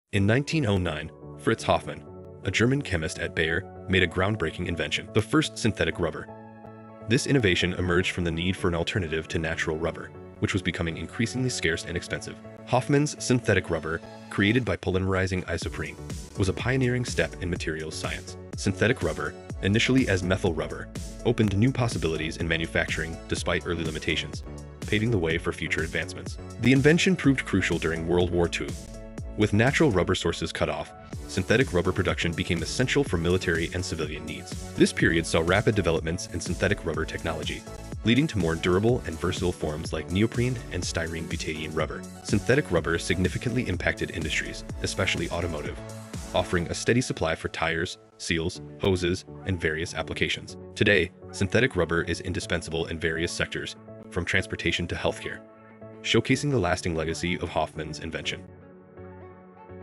Invention of the first synthetic rubber 1909
In 1909, Fritz Hofmann, a German chemist at Bayer, made a groundbreaking invention: the first synthetic rubber, known as methyl rubber. This innovation emerged from the need for a reliable rubber source, as natural rubber supplies were limited and inconsistent.

Hofmann's synthetic rubber was created by polymerizing isoprene, a process that laid the groundwork for future advancements in synthetic rubber technology. This early form of synthetic rubber was a crucial step in developing more efficient and versatile materials.

The impact of synthetic rubber was profound, especially in the automotive industry. It provided a consistent supply of rubber, essential for manufacturing various components like tires, seals, and hoses. Its versatility allowed for customization of properties, catering to a wide range of industrial applications.

World War II highlighted synthetic rubber's strategic importance. With natural rubber sources inaccessible, synthetic rubber production became crucial for military and civilian needs. This period saw rapid advancements, leading to the creation of new types like neoprene and styrene-butadiene rubber.

Synthetic rubber's invention marked a significant milestone in material science, influencing various sectors from transportation to healthcare. Its development not only addressed the limitations of natural rubber but also paved the way for innovative material applications, underscoring its enduring significance in modern industry.